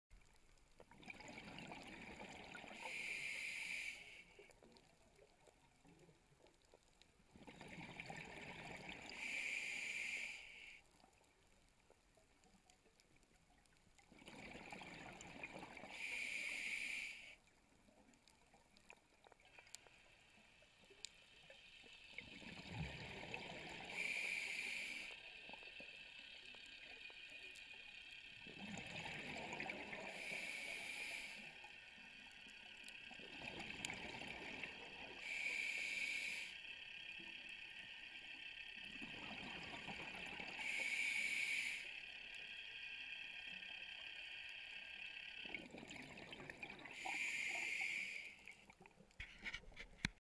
JimJam Dive - South Drop Structure
Example of rock formations on south drop off St John, USVI